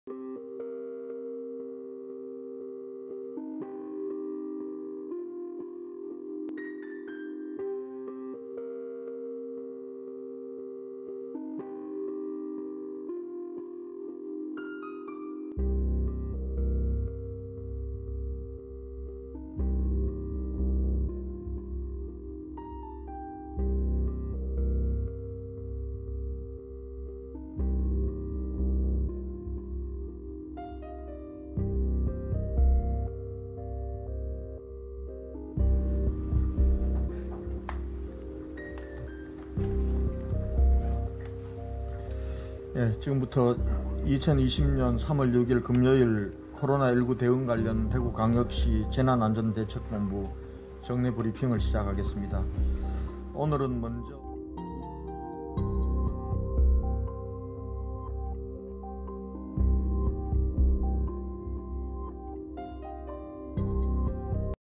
韓國新天地教會捐120億韓元被退回，大邱市長公開拒絕！
韓國新天地教會捐120億韓元被退回，大邱市長公開拒絕！3月5日，新天地教會未經提前協商直接向該機構匯款，其中100億韓元（約合5837萬元人民幣）點名捐給重災區大邱。3月6日，大邱市長公開拒絕新天地捐款並指責部分教徒阻撓防疫。